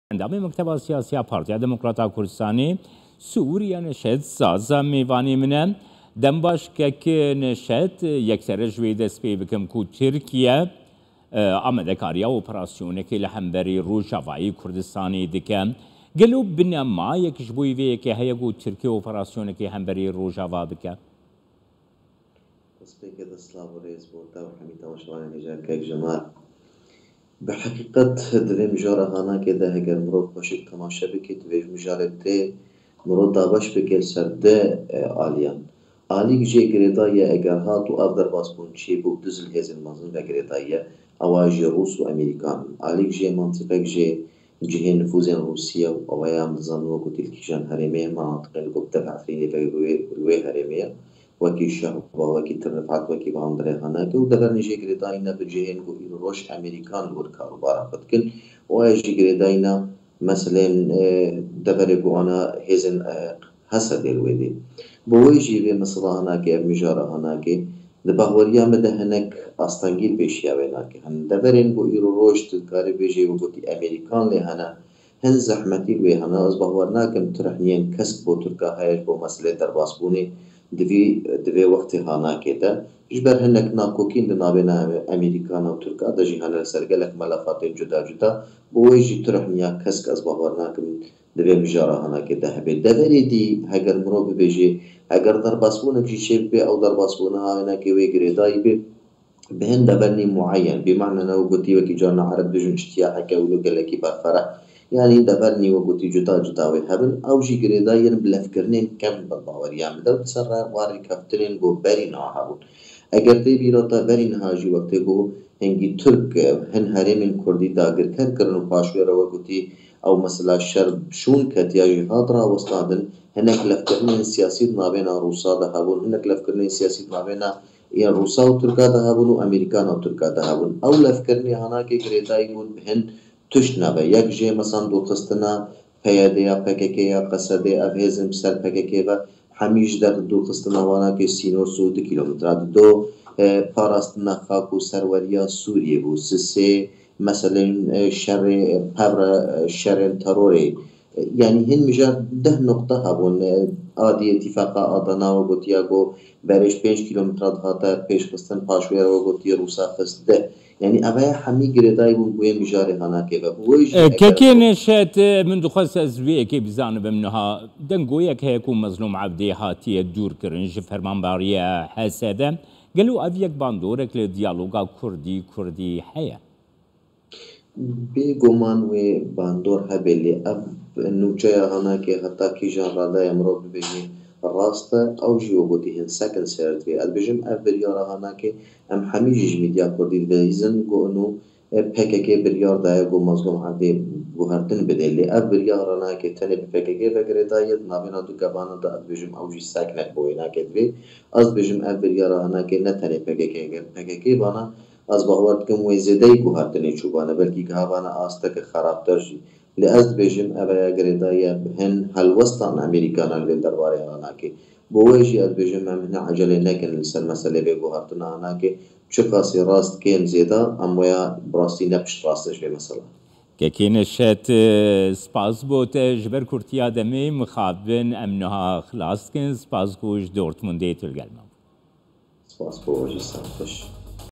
K24 Nûçe: 00:00 Ligel: Neşet Zaza
Endamê Politburo ya PDKS Neşet Zaza,
egera Operasyona Tirkiye hemberî Rojava û mijara Mazlûm Ebdî dinirxîne
06 Mijdar 2921